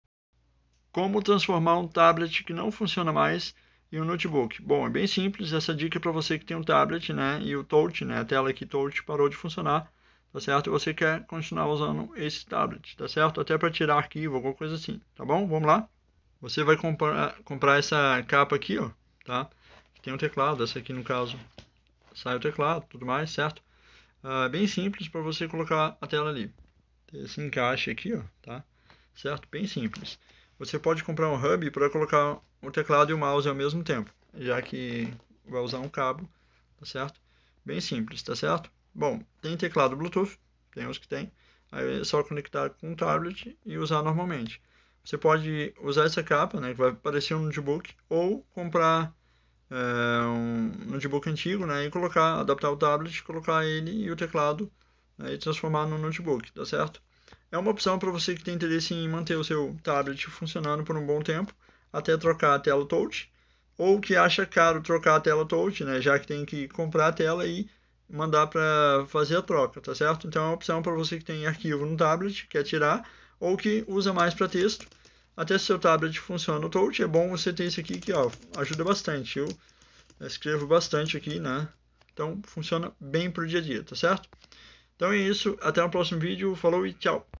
Com transformar um tablet com defeito em um notebook?
Tablet com defeito tem conserto?

Se você quer transformar um tablet usado em um notebook veja como você pode retirar arquivos de um tablet que a tela touch não funciona mais, e ainda transformar seu tablete em um notebook, se você tem um tablet que não funciona, veja essas dicas pra você transformar um tablet que não funciona a tela mais, em um notebook super simples

Vantagens de um tablet com teclado, o sistema operacional Android é super simples e super rápido e muito simples de trabalhar, conexões bluetooth e transferência de arquivos muito rápido

Vale a pena consertar um tablet com defeito no touch
Se você tem um tablet com defeito na tela touch saiba que você pode colocar um teclado no tablet ou uma capa com teclado, e existem vários teclados com bluetooth ou sem